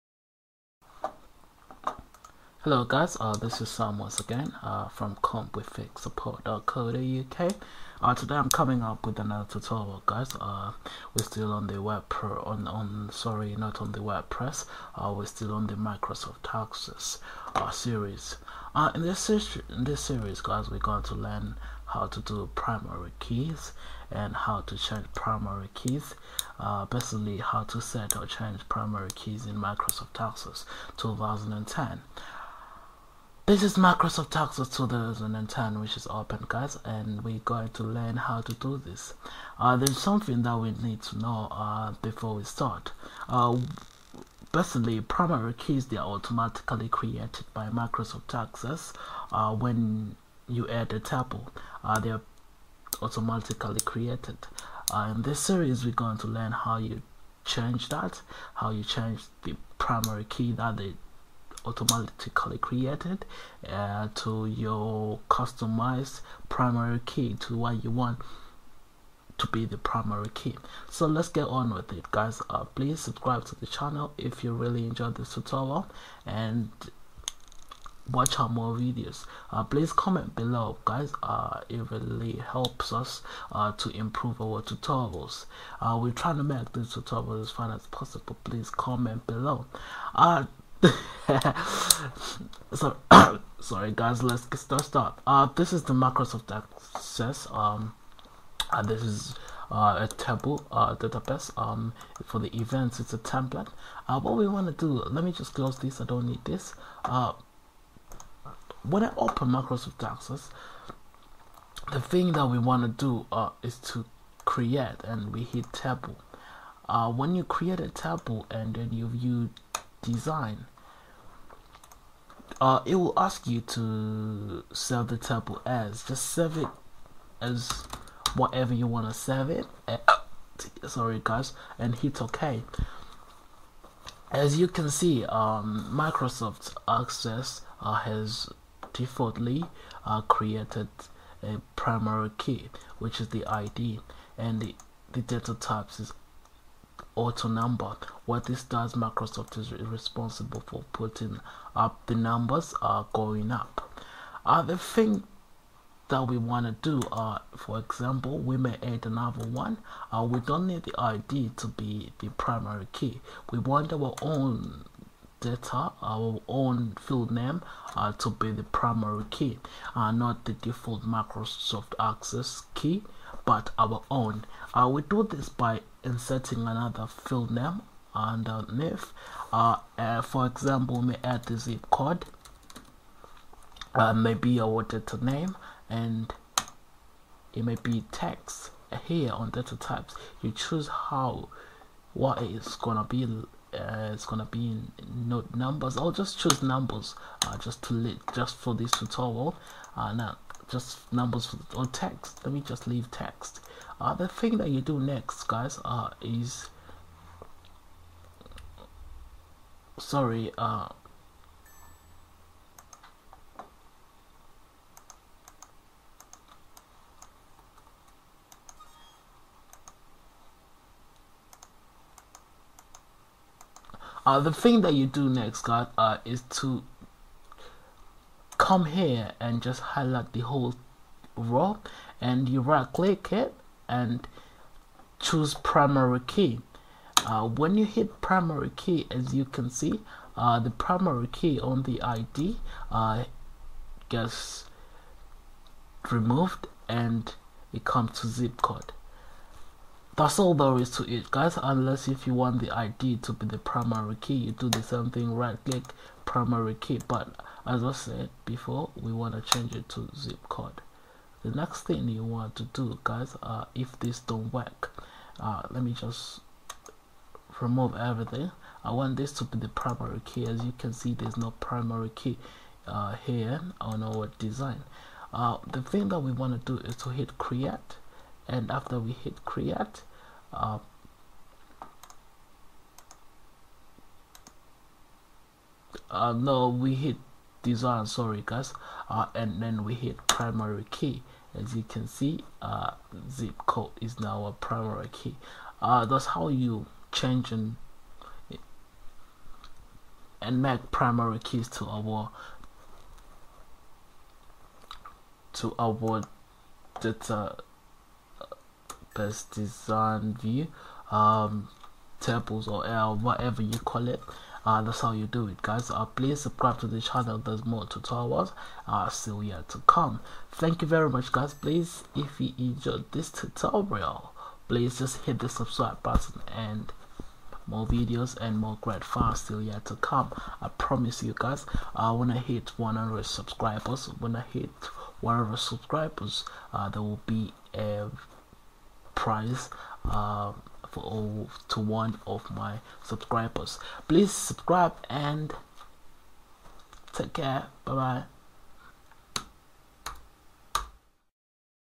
Microsoft Access Tutorial: Creating a Primary Key When You a Table
Hiw in this tutorial I am going to show you how to add primary keys to Microsoft Access 2010.